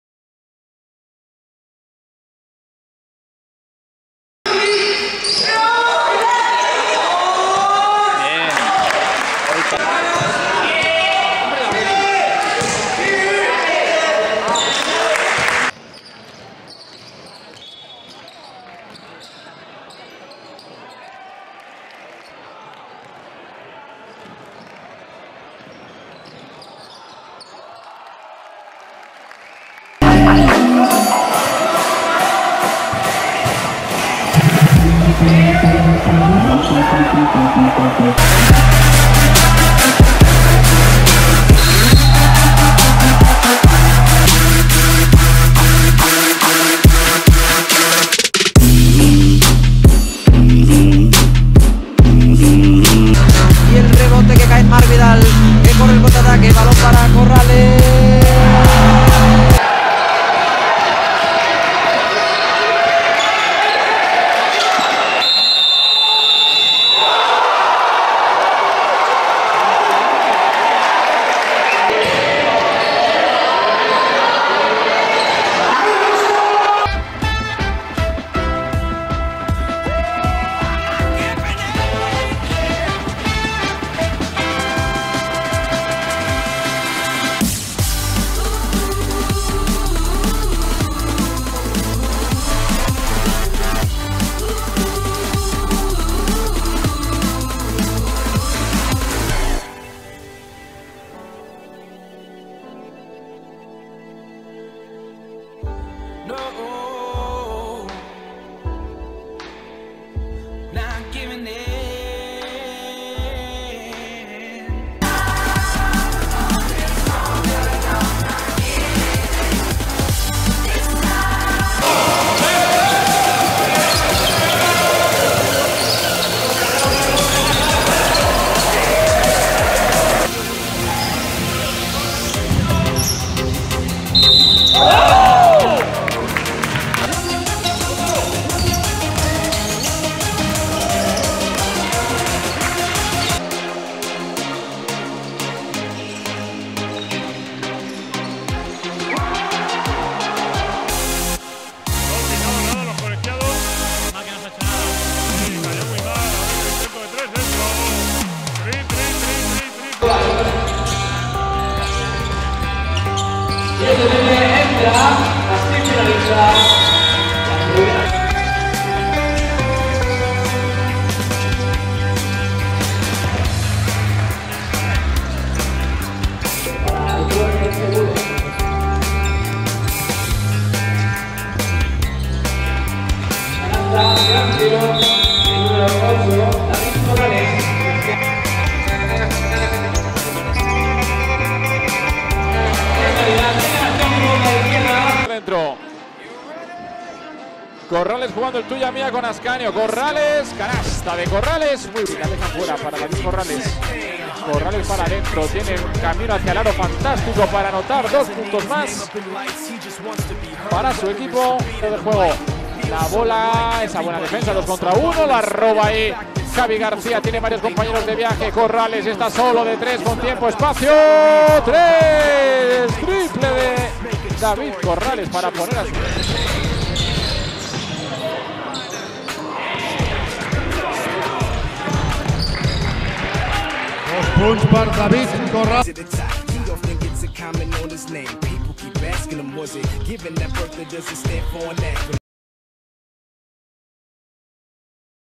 Career Highlights @ Basket Zaragoza David Corrales 01 short version
David Corrales
Point Guard Basket Zaragoza & Anagán Olivar (EBA League)
from 2014 to 2019
Short version